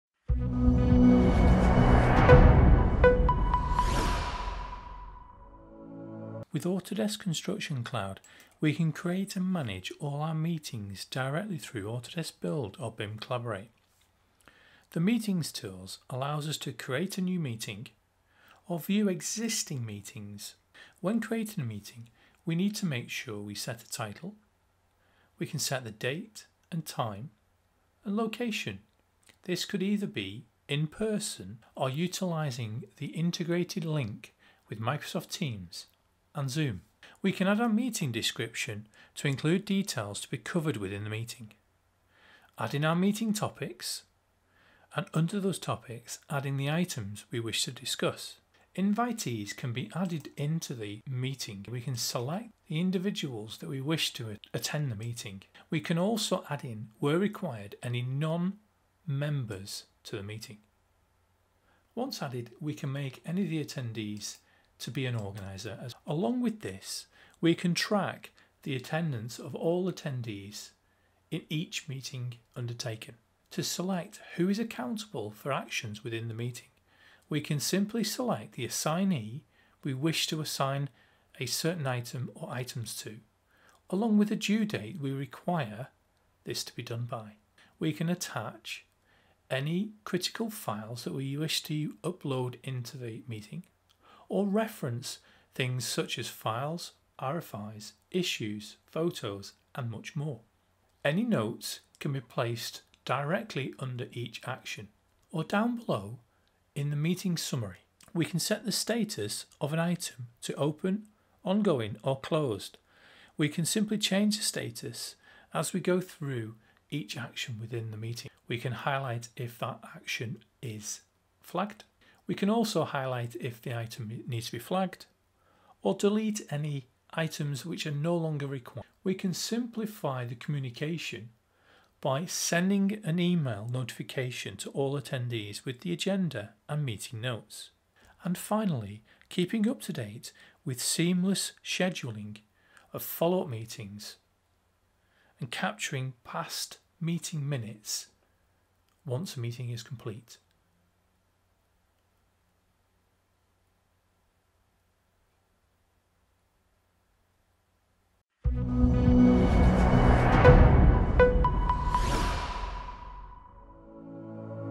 Autodesk Construction Cloud - Autodesk Build Meetings
Meetings are an essential part of any construction project, but organizing and tracking them can be a time-consuming and stressful process. Fortunately, Autodesk Construction Cloud offers a powerful meetings tool that simplifies the process and improves collaboration across teams.
In this video, we'll explore the meetings tool in the Autodesk Construction Cloud and show you how to organize and manage meetings more effectively. We'll walk you through the process of creating meetings, inviting attendees, and sharing important project documents and information. You'll also learn how to track action items and follow-ups from each meeting, ensuring that everyone is on the same page and progressing towards project goals.
With Autodesk Construction Cloud's meetings tool, you can streamline your communication and collaboration, saving time and reducing the risk of errors and misunderstandings. So, if you're looking to improve your construction project management process, join us as we dive into the benefits of using meetings in Autodesk Construction Cloud.